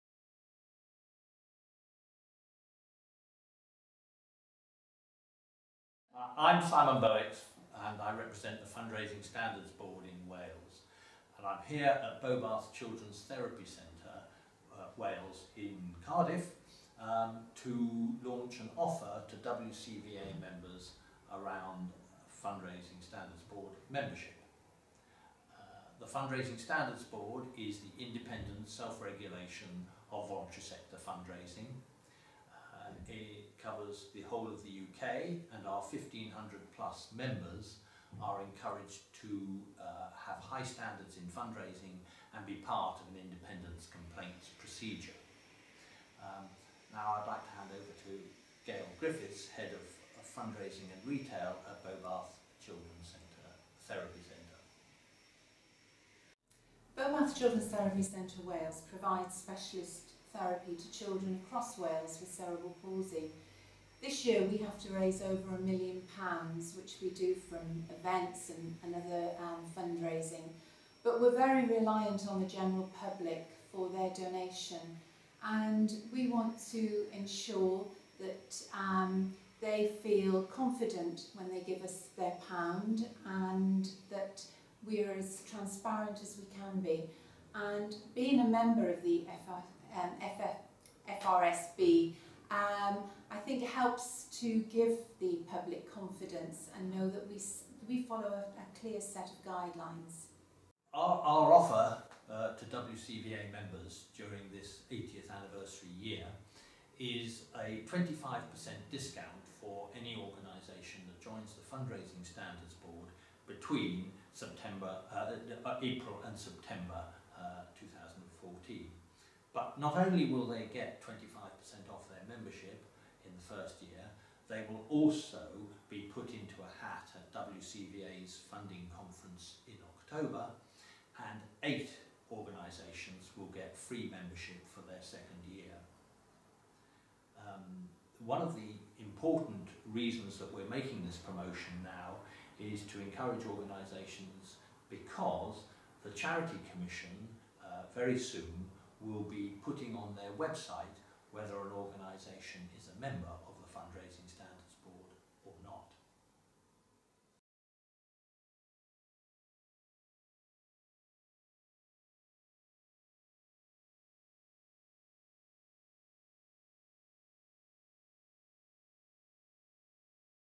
The Fundraising Standards Board - a special offer for WCVA members
WCVA members can get a 25 per cent discount on a year's membership of the Fundraising Standards Board, and the chance to win their second year free - watch the video to find out more, and hear Simon Boex of FRSB discussing why it's more important than ever to join FRSB as the Charity Commission gets set to list FRSB members on their website. And Gail Griffiths of Bobath Children's Therapy Centre Wales tells how FRSB membership gives the public extra confidence in their charity.